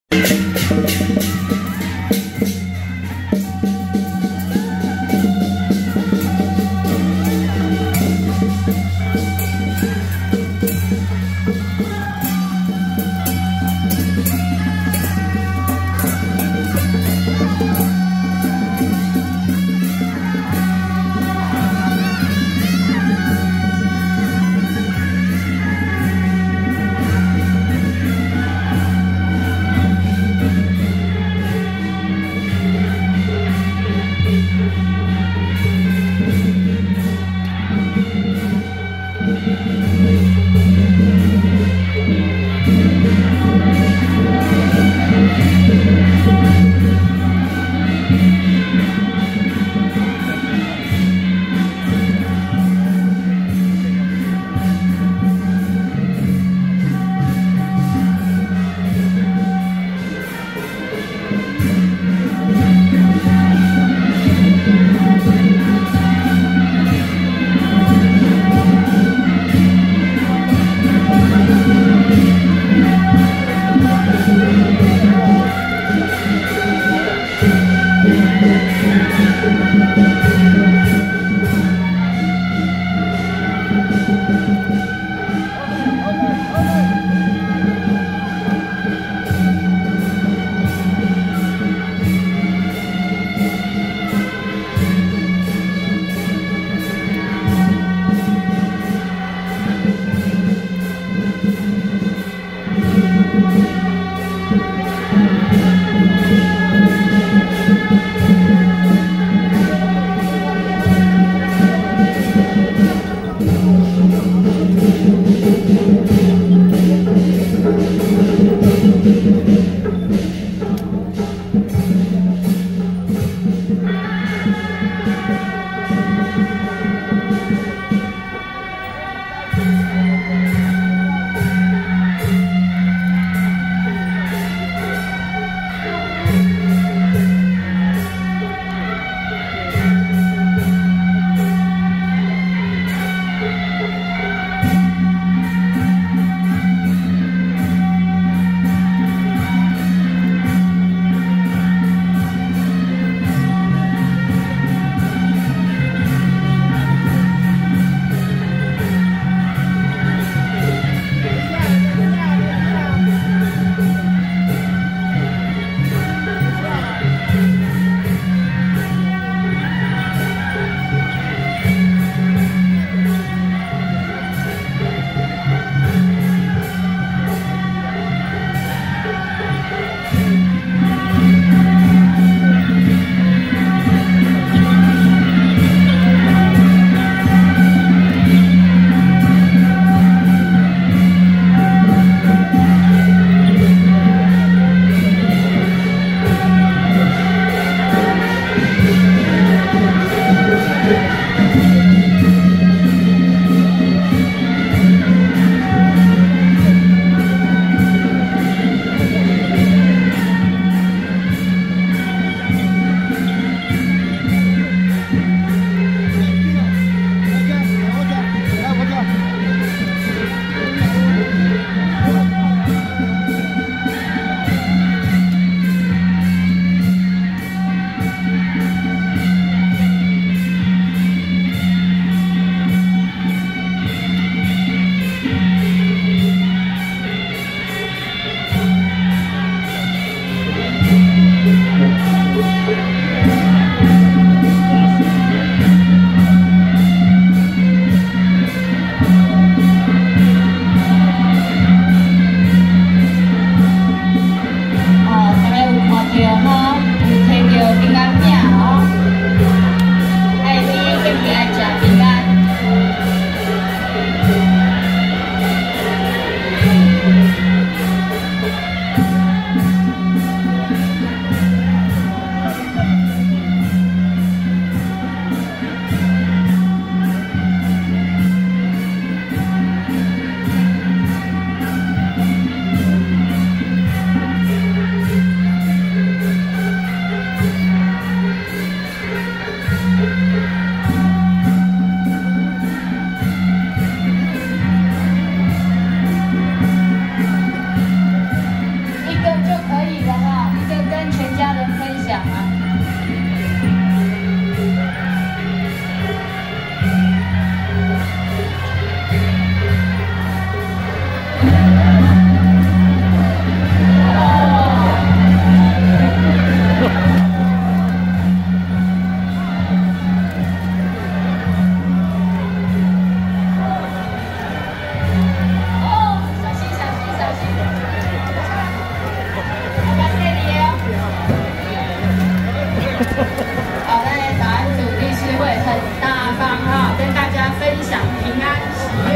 【大龍峒保安宮保安祖-神將團＆保安祖 聖駕】~113大龍峒保安宮恭祝保生大帝聖誕遶境(保生文化祭)
淡水國隆社-北管＆大龍峒保安宮保安祖-神將團＆保安祖 聖駕